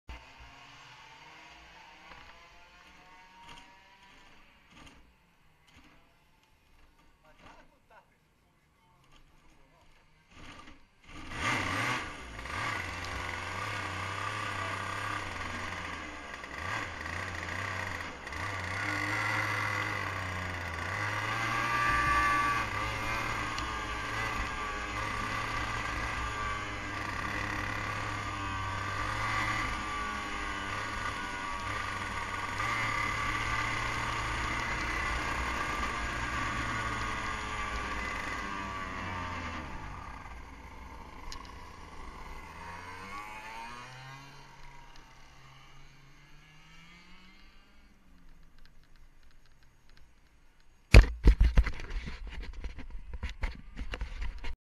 in pista con il gokart ( mi si è ingrippato il pistone :(
bella a tutti ragazzi e benvenuti in un nuovo video. in quest video sono andato alla pista di gokart ( Curno ) bergamo. spero che il video vi piaccia iscrivetevi al canale commentate e buona visione